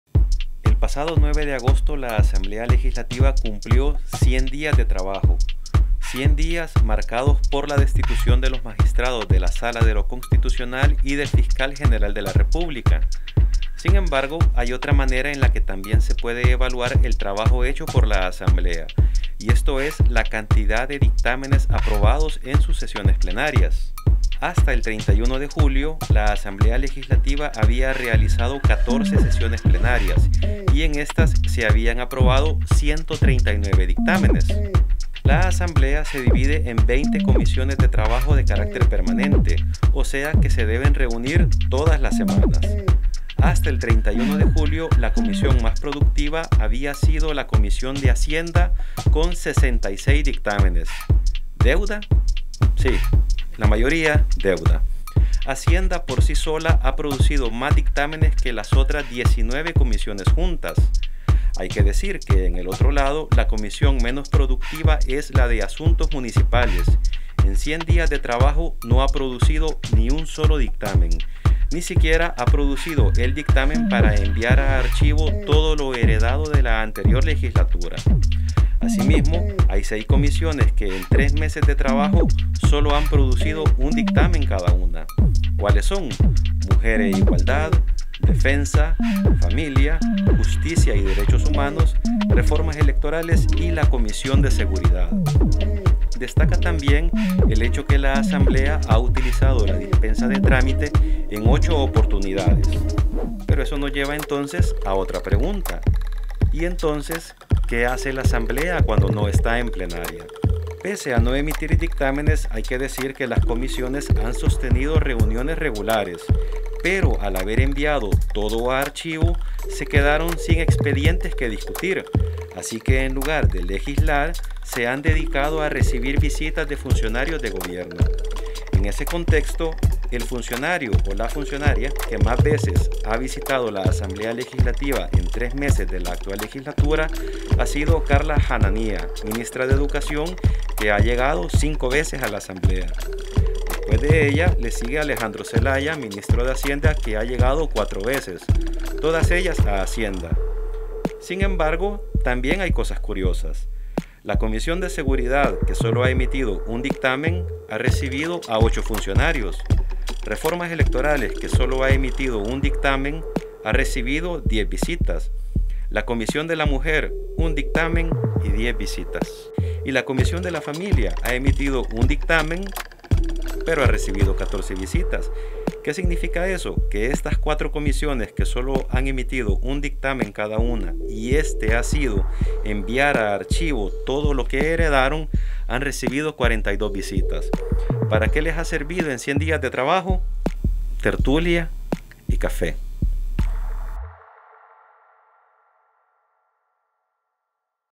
¿Qué hace la Asamblea Legislativa cuando no está en plenaria? Comisiones con baja productividad
En sus primeros 100 días, la Asamblea aprobó 139 dictámenes. De estos, 66 fueron emitidos en comisión de hacienda. Por si sola, esta emitió más que las otras 19 comisiones juntas.